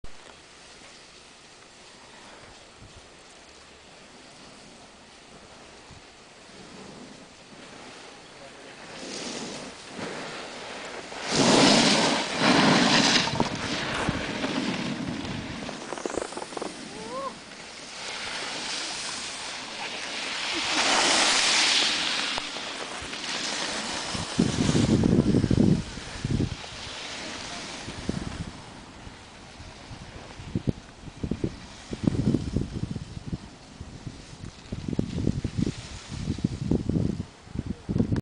Skiing in La Thuile
Kasia and Jörn skiing in La Thuile